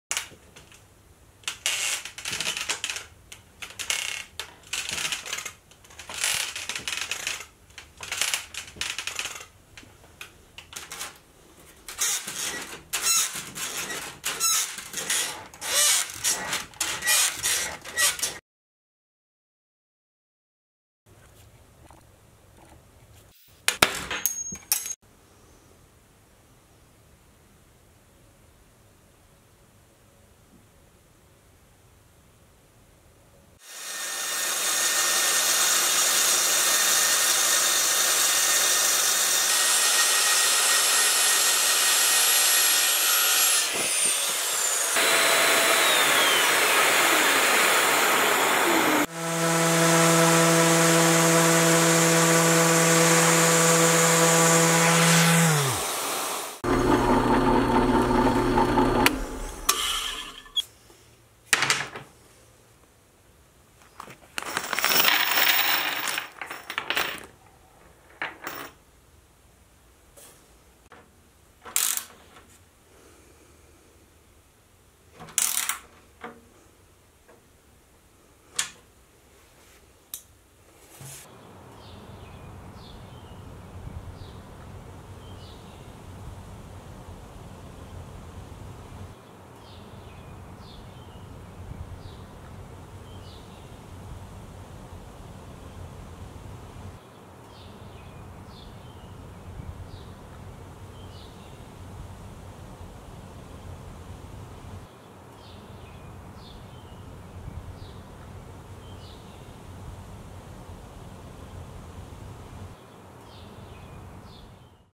My Corona time project
This was my project in the time for Corona, replacing the wood and hardware on an old swing bench.